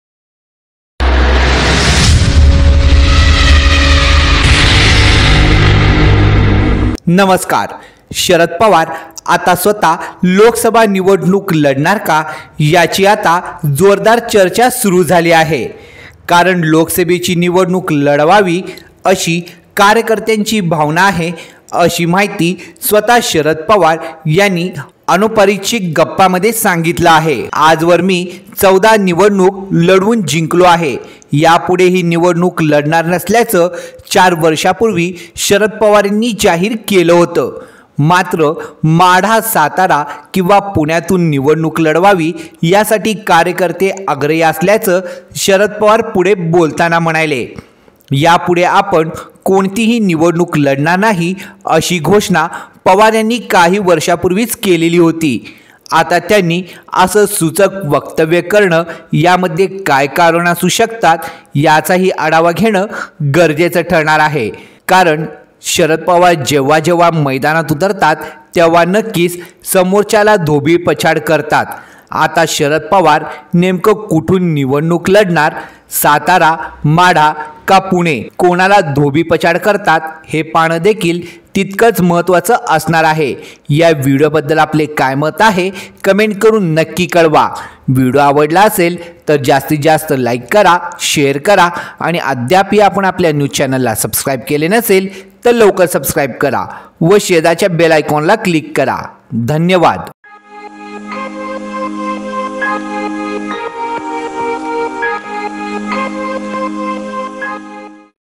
🔴 शरद पवार मैदानात शरद पवार या ठिकाणाहून लोकसभा लढणार? | Sharad Pawar
🔴 शरद पवार मैदानात शरद पवार या ठिकाणाहून लोकसभा लढणार?